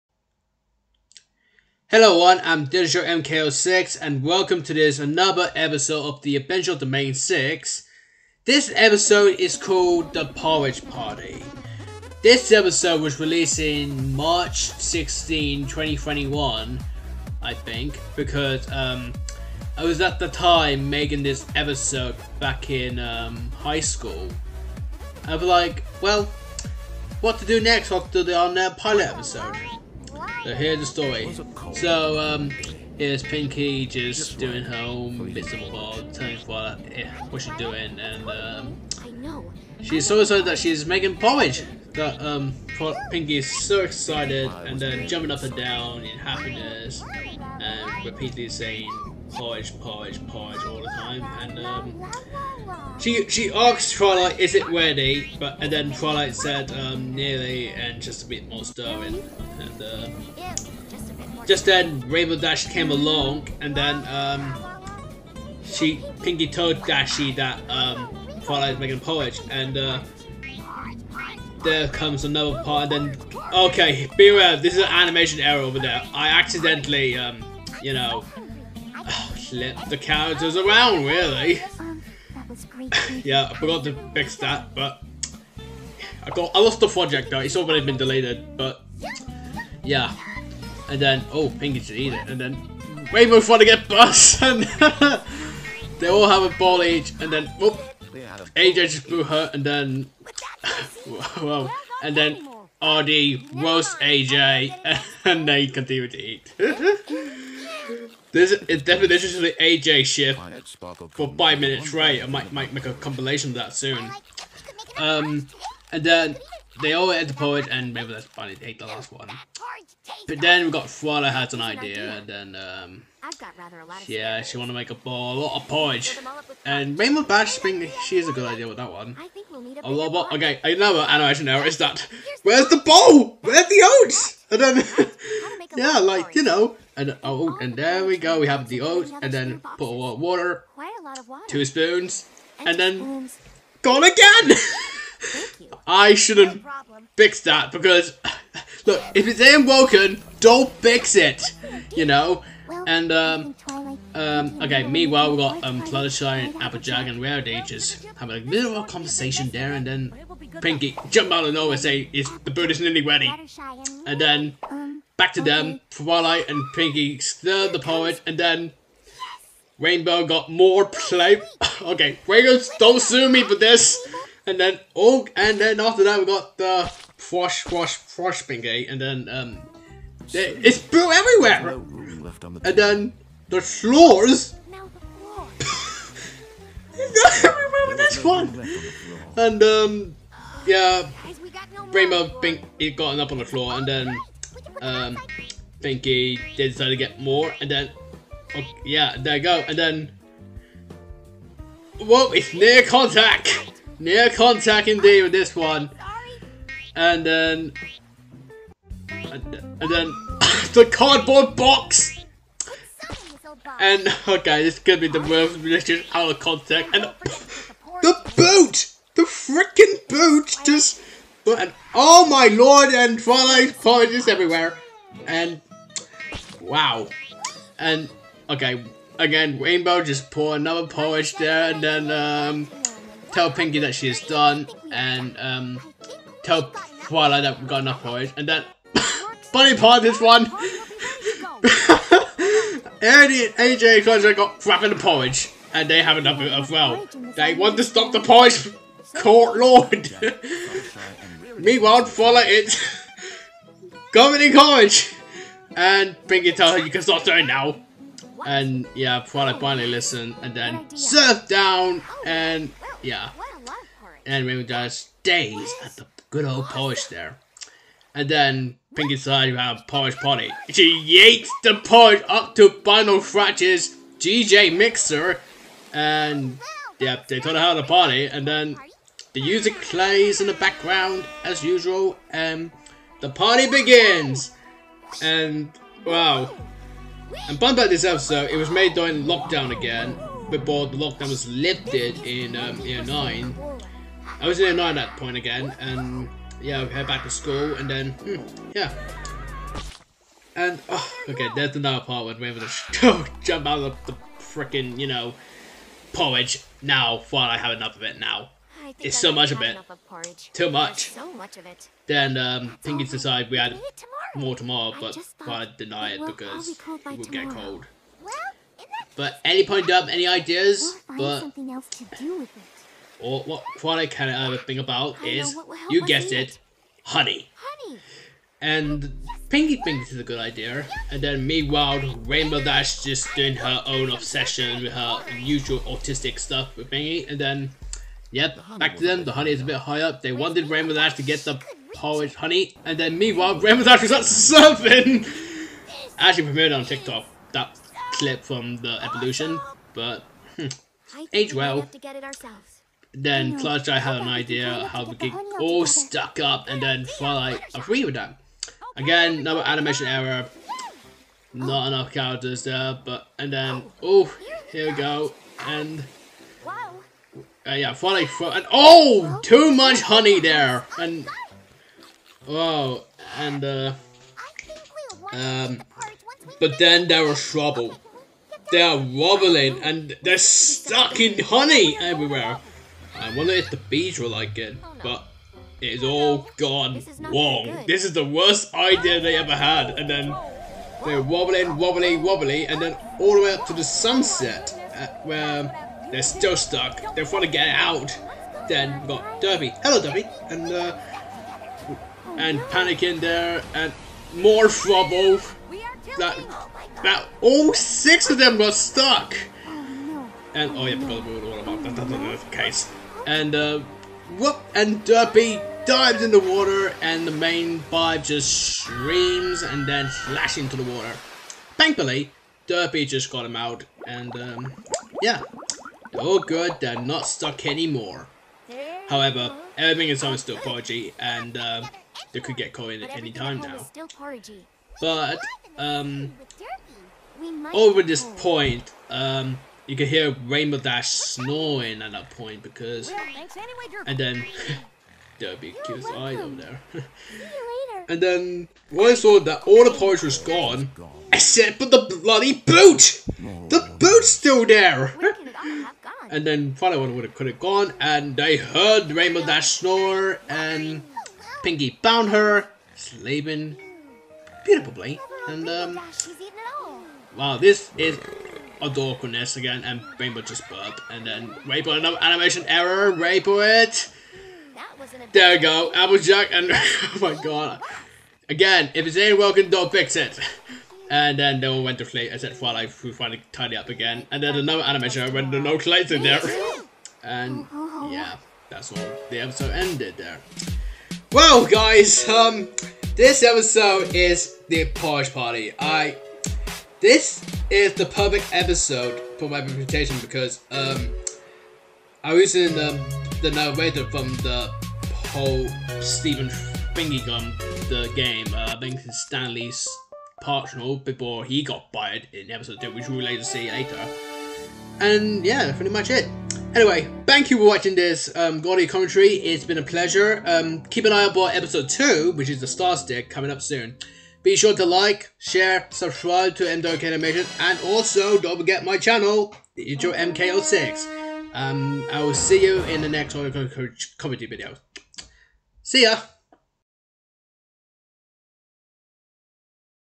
The Adventure of the Mane 6 - S01E01 - The Porridge Party (Audio Commentary)
Join @digitalmk06  in this delightful audio commentary as we dive into the inaugural episode of "The Adventure of the Mane 6" – S01E01, aptly titled "The Porridge Party."

*PLOT*
The Mane 6 make lots of porridge to warm them up on a cold and gloomy day. They make so much that it fills all the bowls all over the island and starts to get everywhere. Pinkie Pie decides they should have a porridge party to use it all up.

*DESCRIPTION*
The Mane 6 are back! And this time, they are stranded on a desert island.  They share friendship of friendly hilarious characters. Join Twilight Sparkle, Rainbow Dash, Pinkie Pie, Applejack, Fluttershy and Rarity on their biggest adventure yet on the island.

All character were voiced by 15.ai

Note: No AI voices are harmed during the making of this episode.

My Little Pony: Friendship is Magic belongs to Hasbro Studios